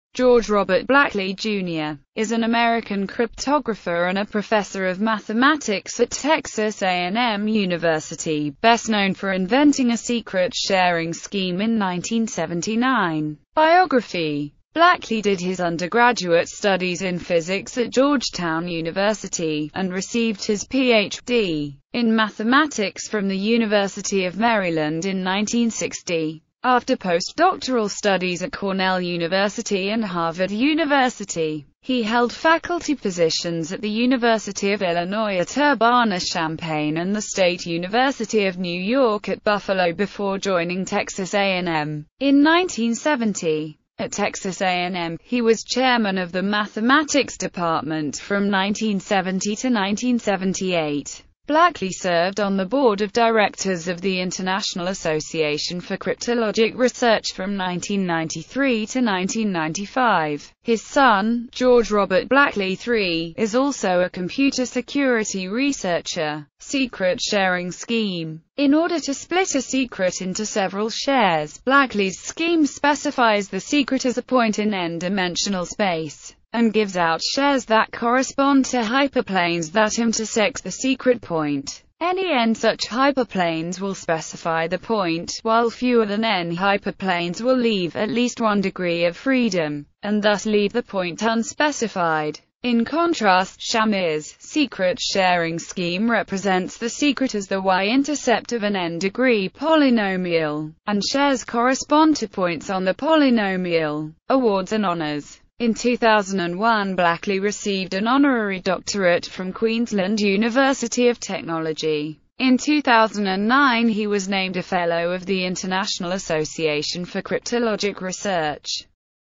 George Blakley
George Robert (Bob) Blakley Jr.is an American cryptographer and a professor of mathematics at Texas A&M University, best known for inventing a secret sharing scheme in 1979.